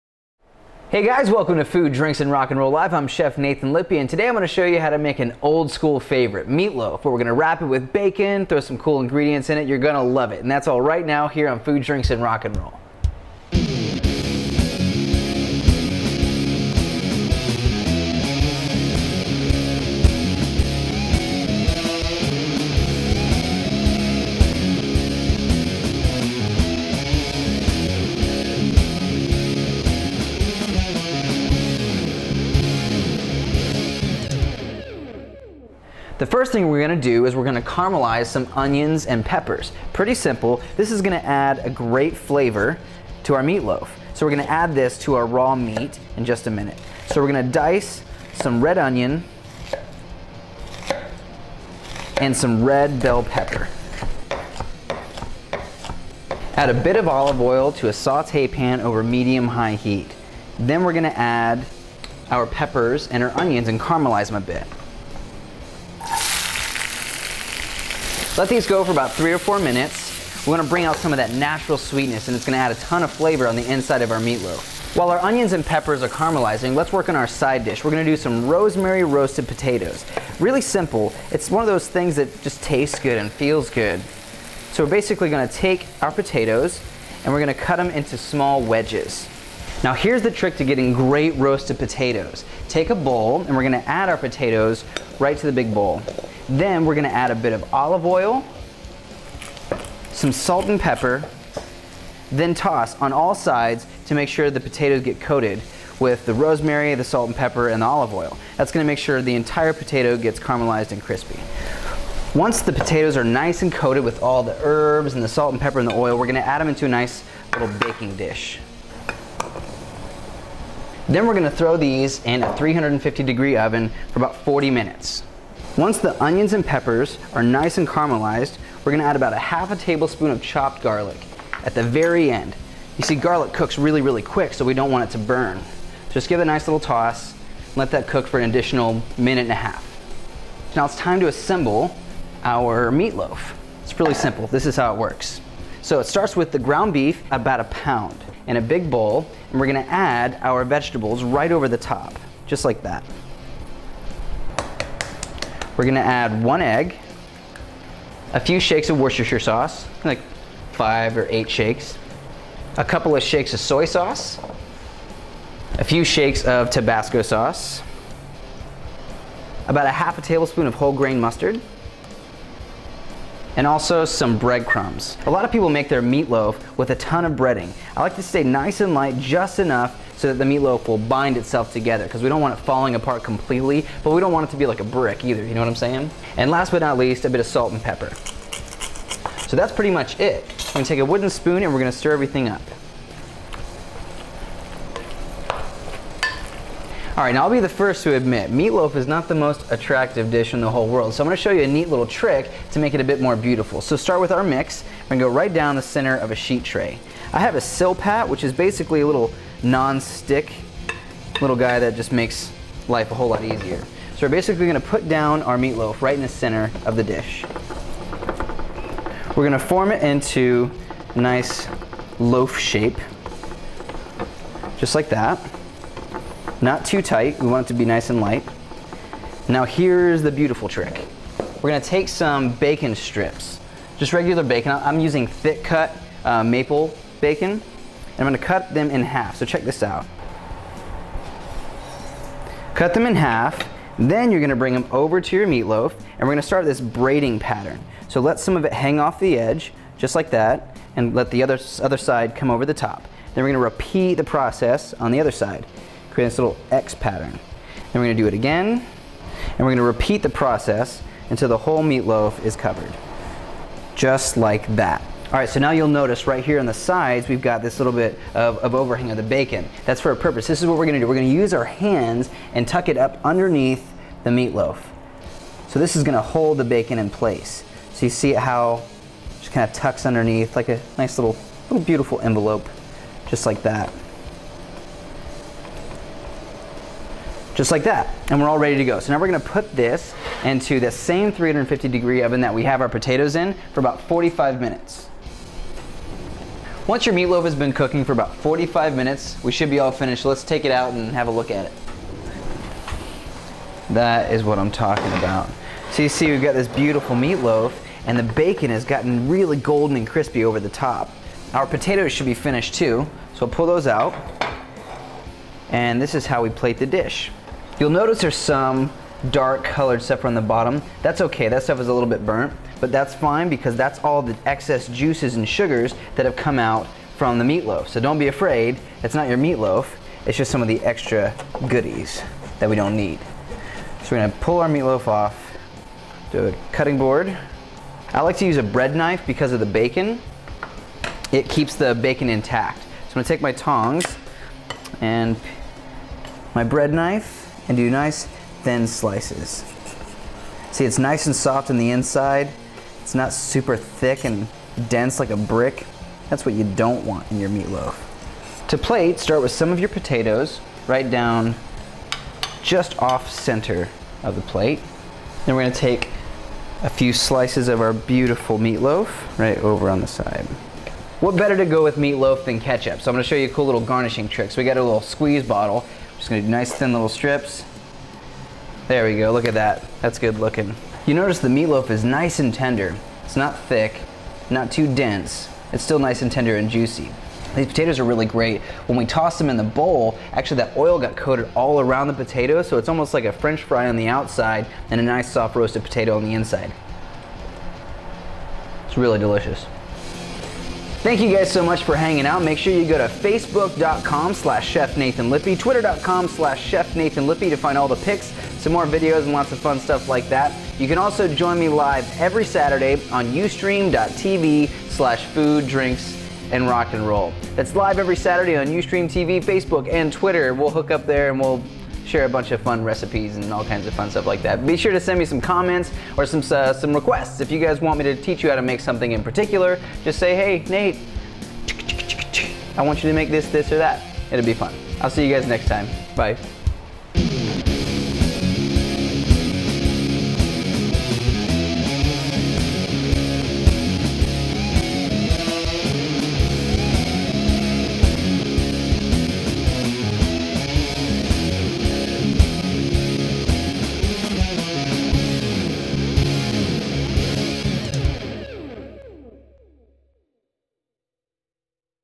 Bacon Wrapped Meatloaf with Rosemary Roasted Potatoes - Nathan Lippy
Meatloaf is a classic dish and this is my spin on it. Bacon makes everything better especially meatloaf! In this clip, I'll show you how to take meatloaf to the next level. Momma's meatloaf simply won't cut it anymore... I hope you dig my meatloaf clip.